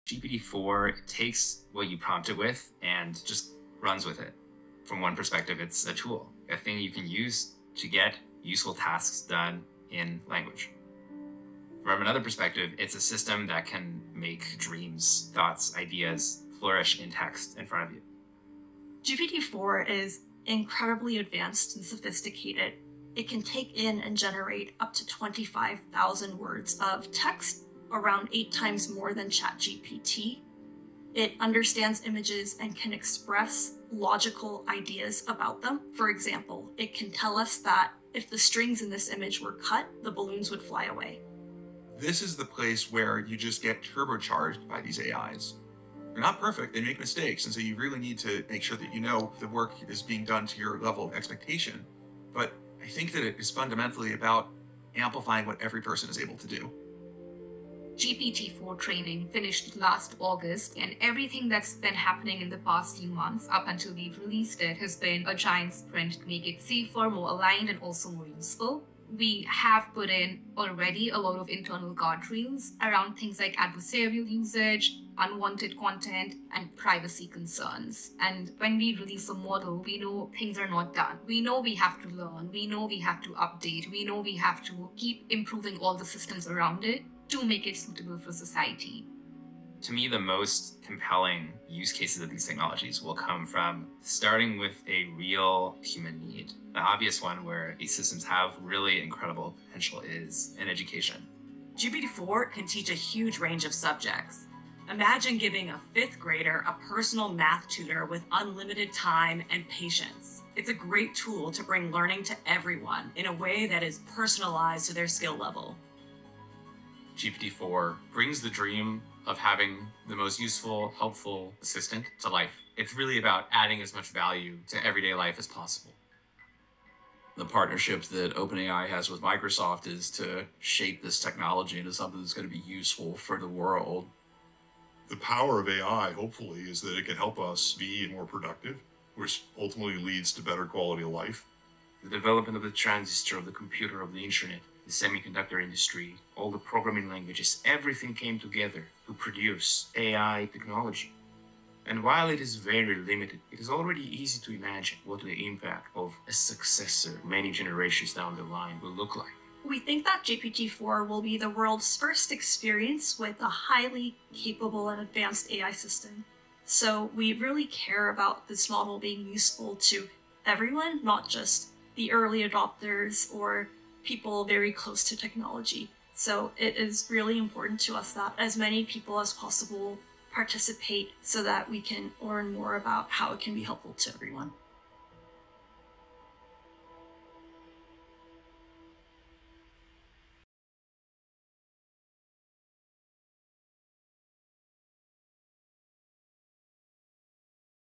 GPT4 et c'est énorme ce que l'intelligence peut faire, elle serait capable de raisonner !
GPT4 vient de sortir, voici toutes les possibilité que l'on peut faire : analyse de texte, analyse de photo, analyse de documents. Il est même capable d'expliquer des mêmes.